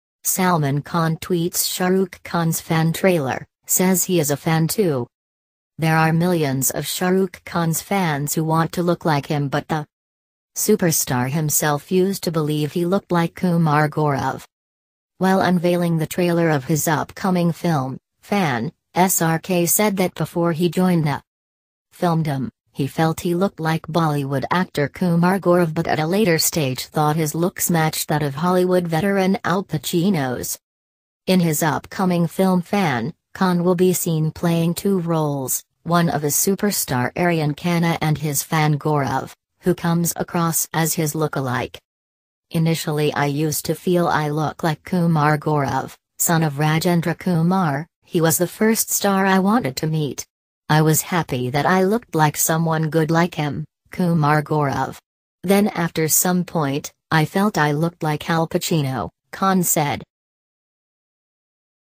Salman Khan tweets Shah Rukh Khan’s Fan trailer, says he is a fan too
Salman Khan tweets Shah Rukh Khan’s Fan trailer, says he is a fan too  - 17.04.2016